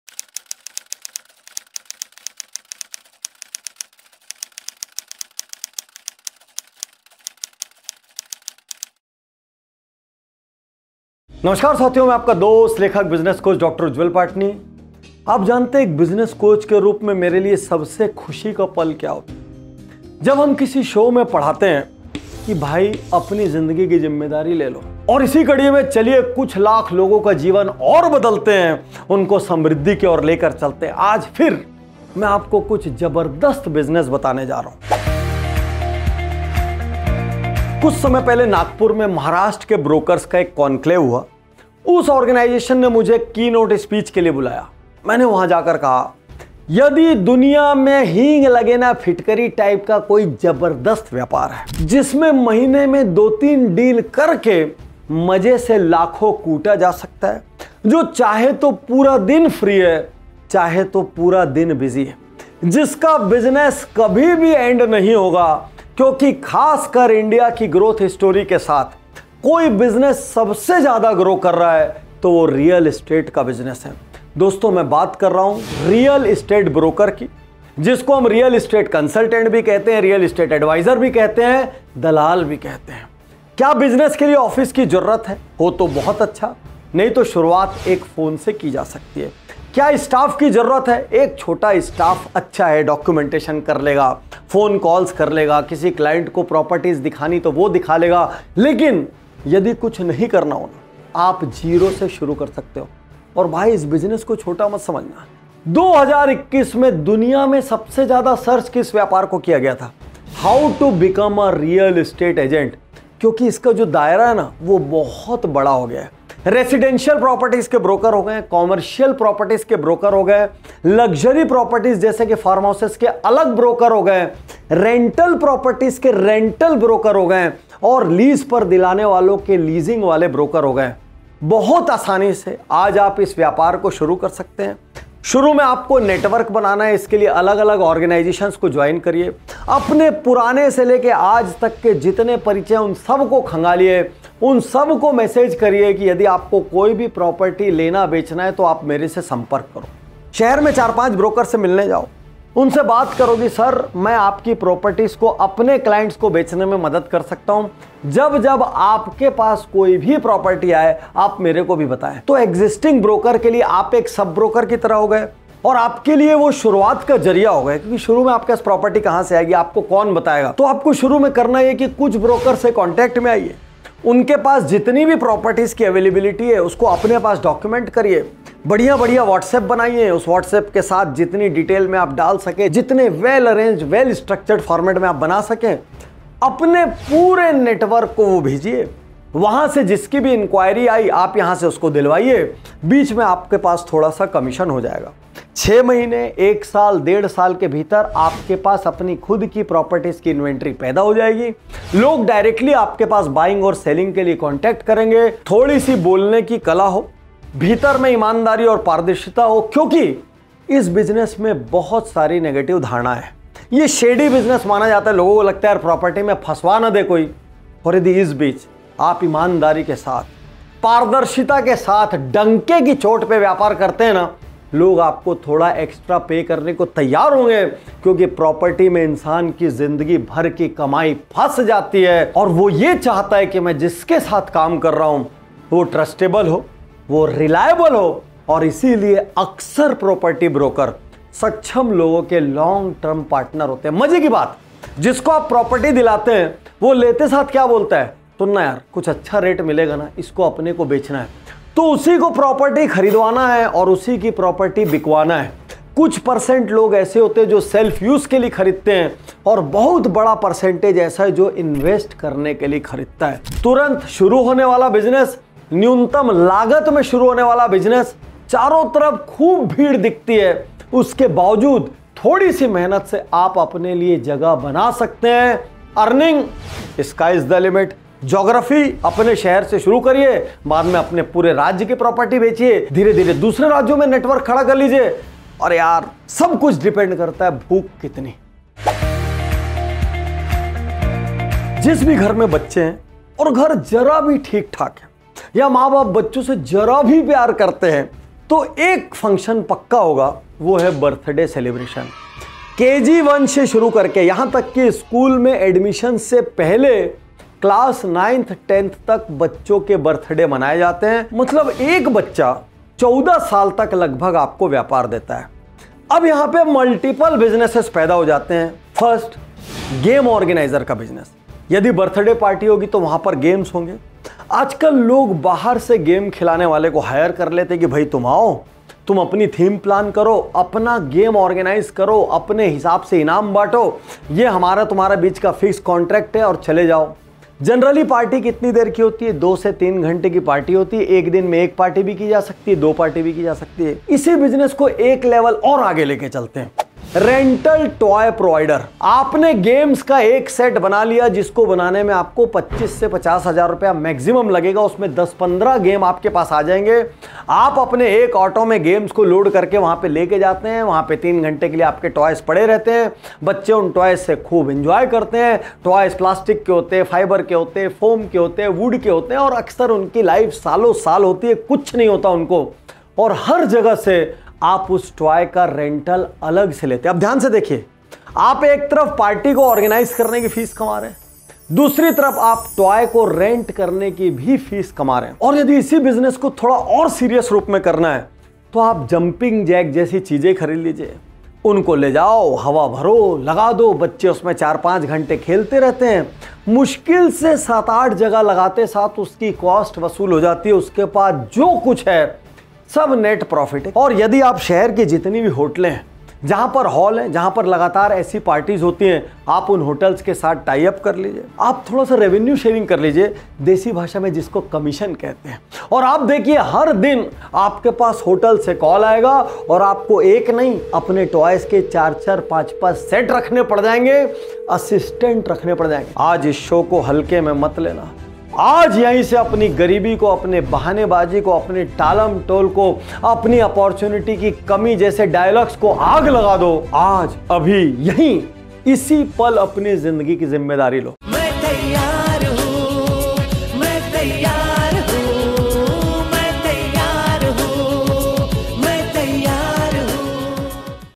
2 व्यापार | अकेले करो, पैसा कमाओ | Start your Business Now | Dr Ujjwal Patni
In this highly motivational video, Dr. Ujjwal Patni delves into his keynote speech at the Maharashtra brokers' conclave in Nagpur, sharing profound insights into the immense potential of the real estate business.

Discover how this lucrative and flexible career can be initiated with minimal investment, even just a phone. Dr. Patni underscores the various specializations within real estate, including residential, commercial, luxury, rental, and leasing properties.

Learn effective networking strategies-from reaching out to acquaintances to collaborating with local brokers as a sub-broker. With these strategies, you can establish yourself in the competitive real estate market and achieve financial success.

In this video, you will learn:
✅ The vast potential of the real estate industry.
✅ Starting with minimal investment and expanding your business.
✅ The pivotal role of honesty and transparency in real estate transactions.
✅ Innovative business ideas to diversify your income streams.

To attend Dr. Ujjwal Patni's courses, call: ✅ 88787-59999
Watch the introduction of Dr. Patni: ✅ • Profile: Dr Ujjwal Patni